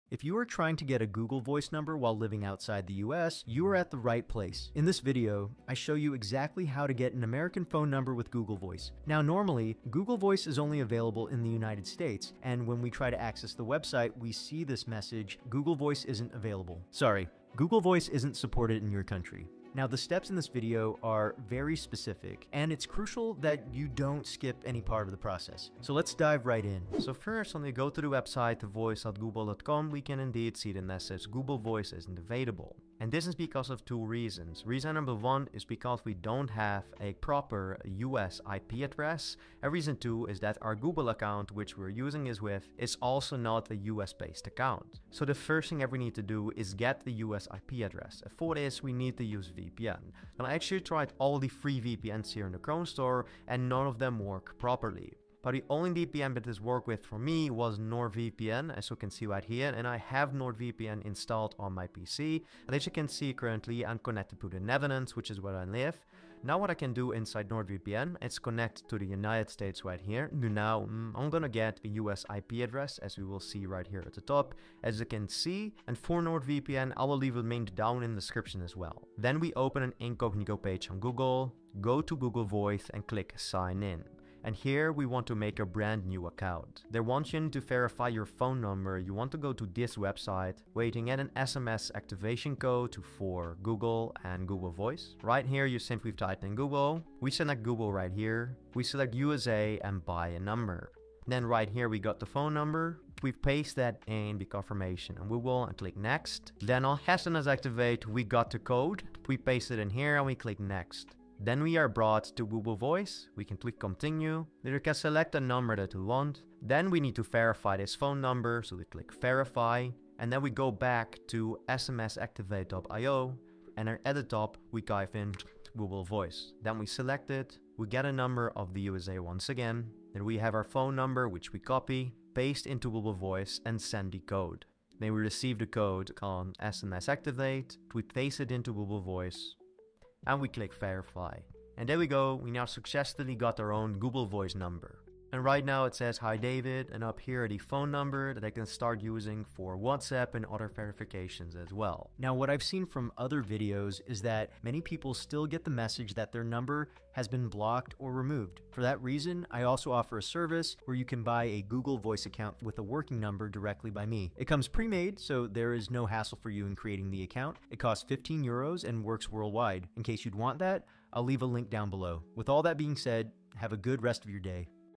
How to Get Google Voice Number Outside the US | Get Google Voice in Any Country 2025
Want an american Google Voice phone number for cold calling, whatsapp and verifications when outside the USA? Thisvideos shows you exactly how to get a Google Voice number outside the United states, making it super easy to create your own free US phone number